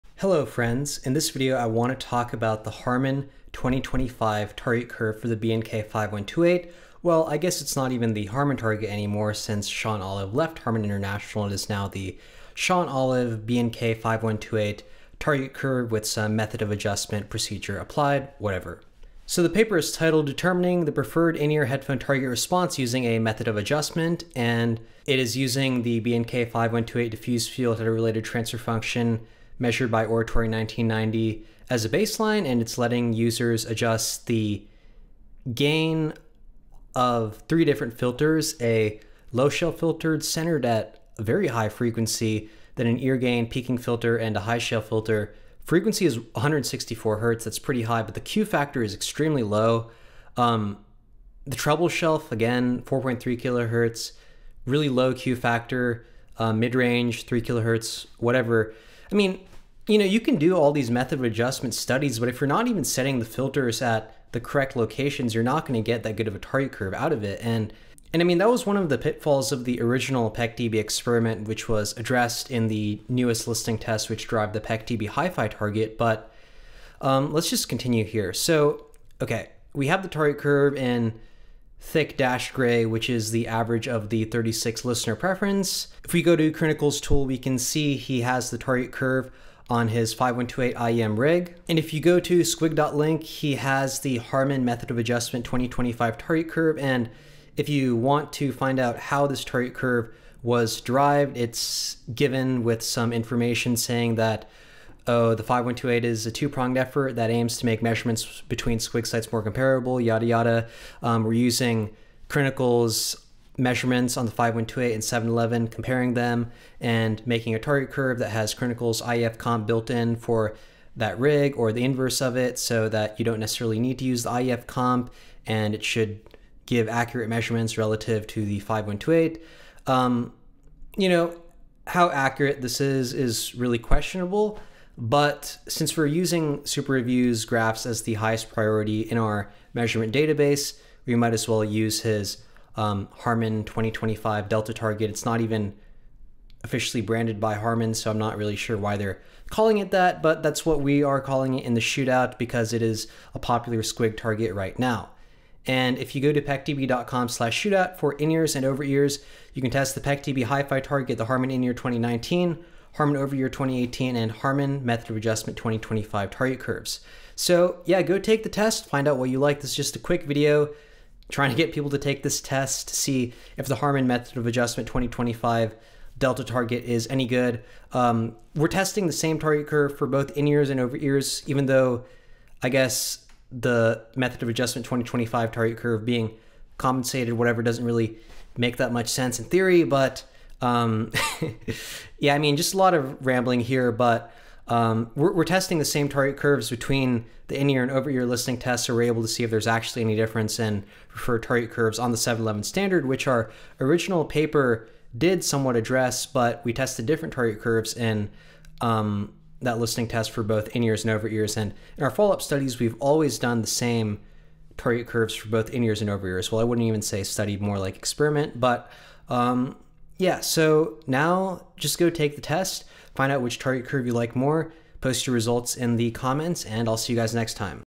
2025 Harman 5128 Target Officially Tested!
Is the Harman 2025 target curve developed on the B&K 5128 actually any good?

Recommended Audio Equipment:
Open-Back Headphones
In-Ear Headphones
Portable Bluetooth Speaker
Studio Monitors